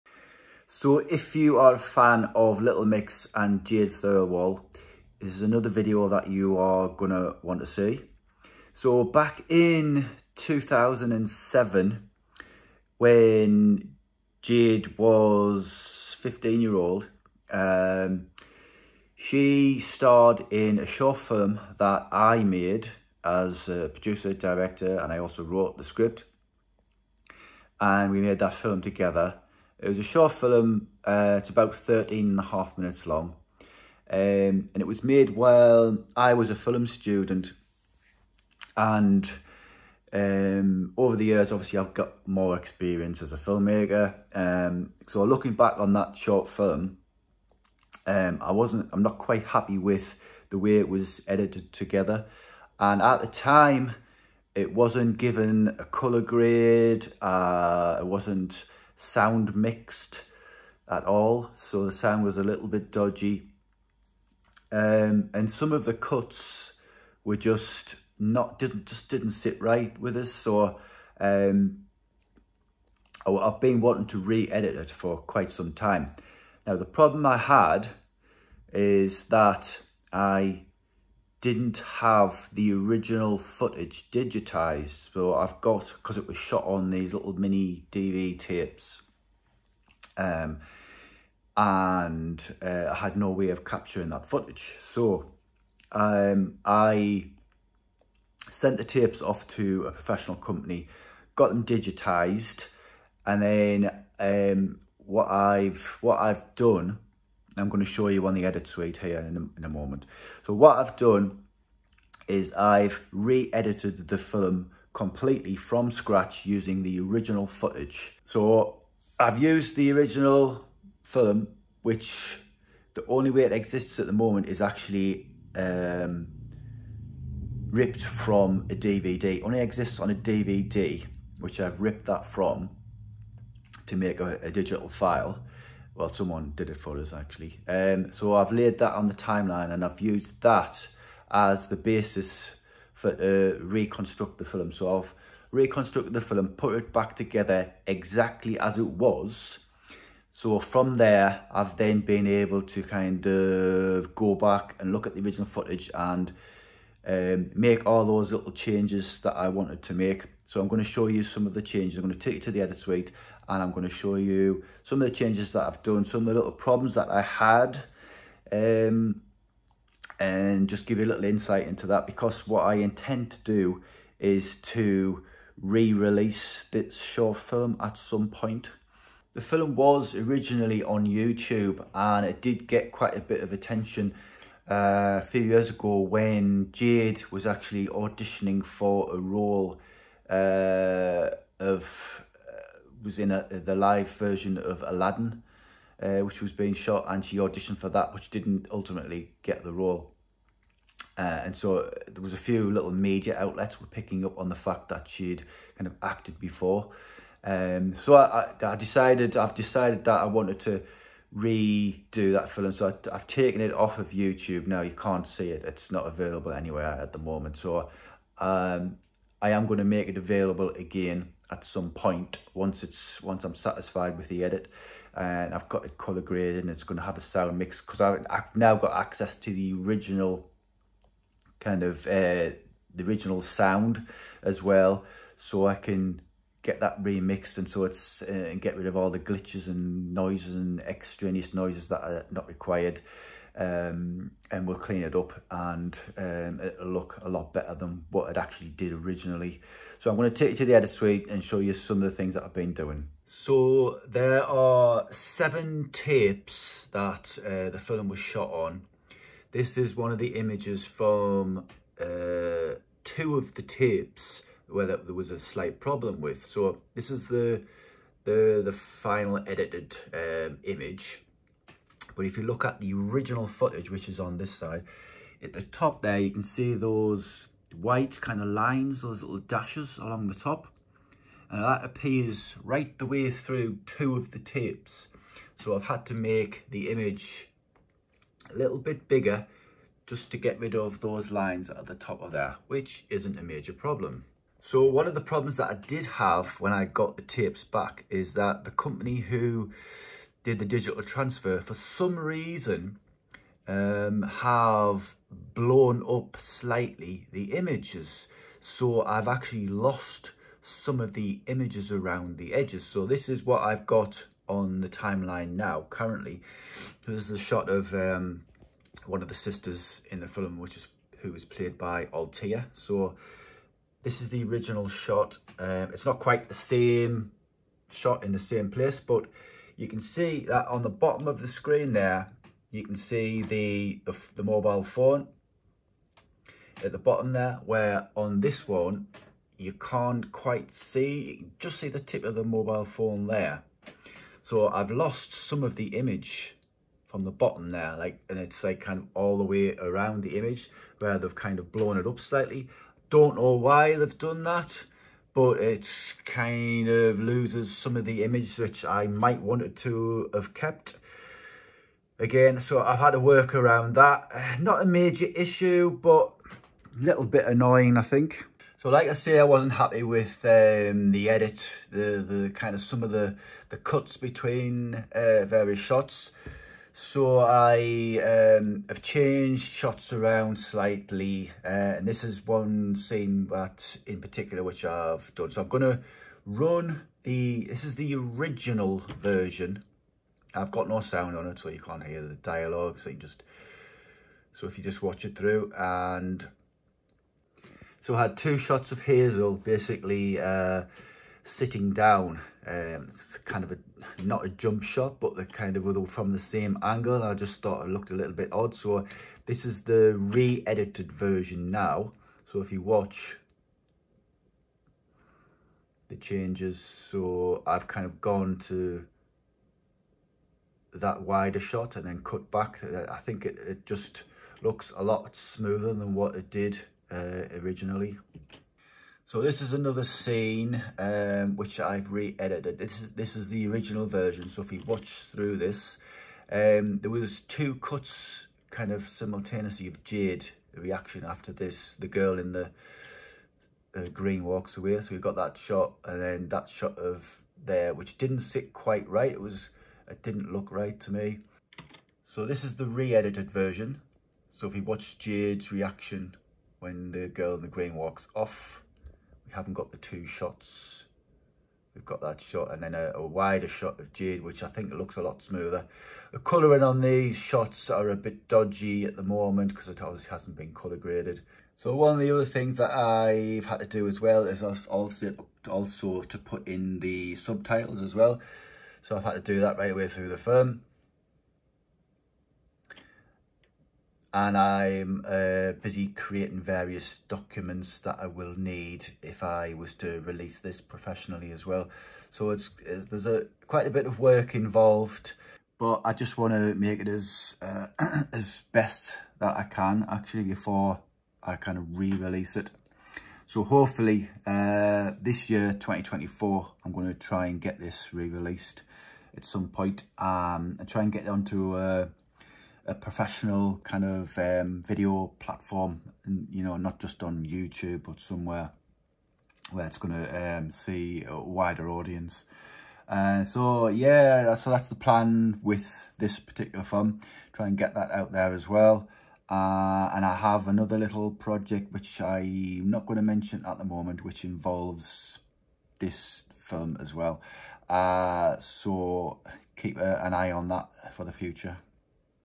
'Skin' - Re-editing Jade Thirlwall's only film
A short video update on the re-edit of 'Skin', the short film starring Jade Thirlwall.